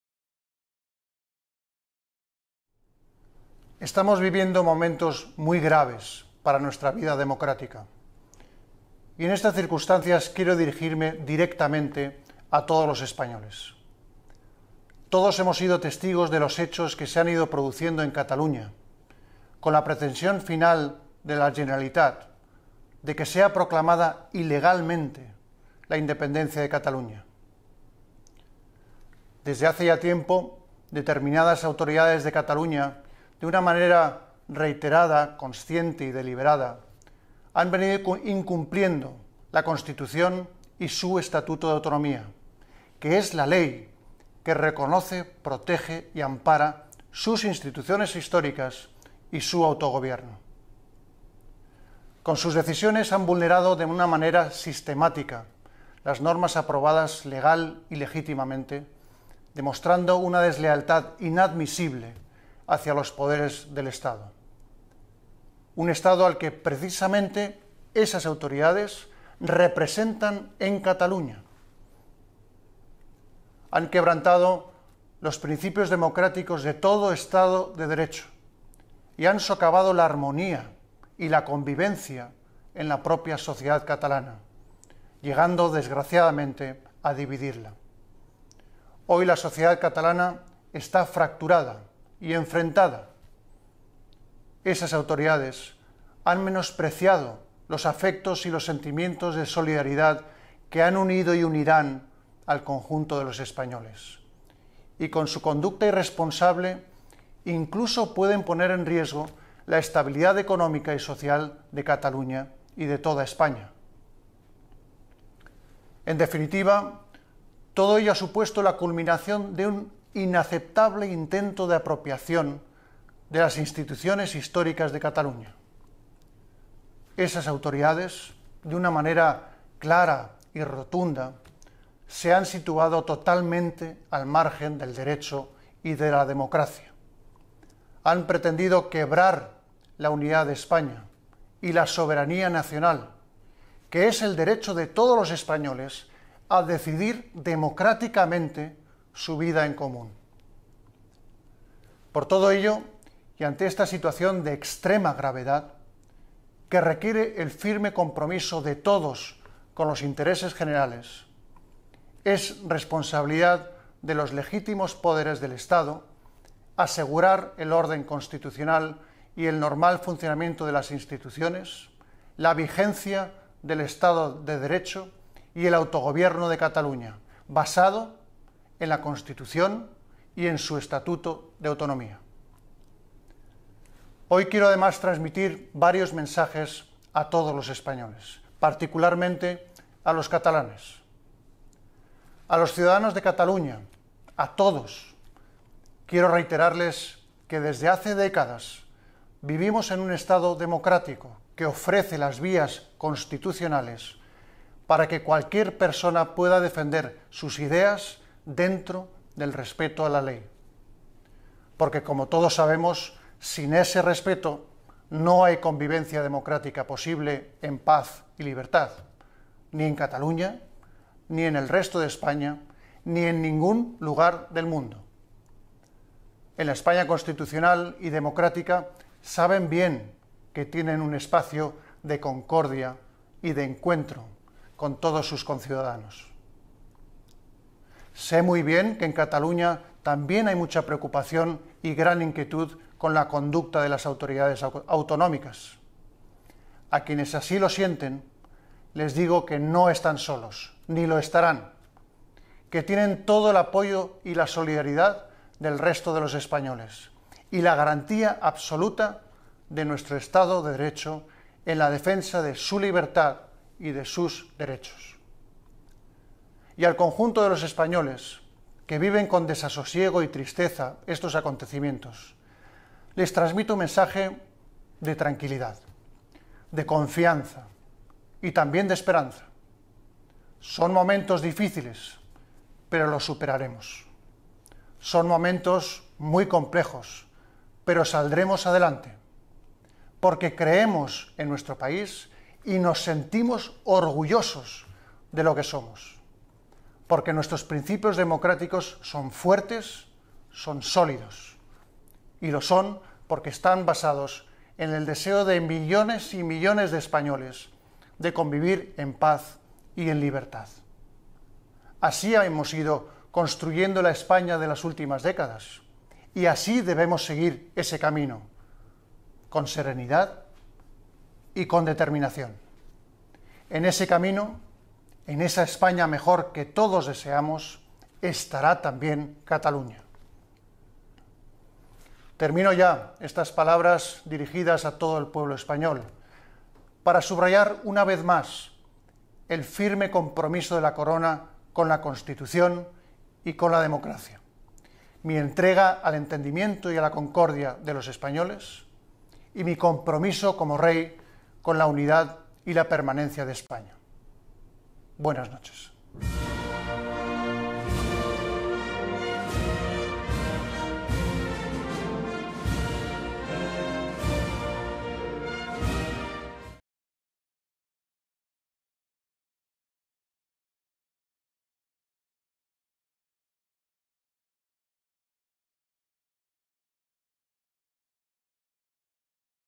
Mensaje de Su Majestad el Rey Felipe VI de España el 3 de octubre de 2017
Mensaje de Su Majestad el Rey Felipe VI de España el 3 de octubre de 2017 hablando del Referéndum en Cataluña del 1 de octubre y la actuación policial española.

Casa de Su Majestad el Rey de España - Archivo Multimedia - Mensaje de Su Majestad el Rey Palacio de La Zarzuela. Madrid, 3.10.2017

Mensaje de Su Majestad el Rey Palacio de La Zarzuela. Madrid, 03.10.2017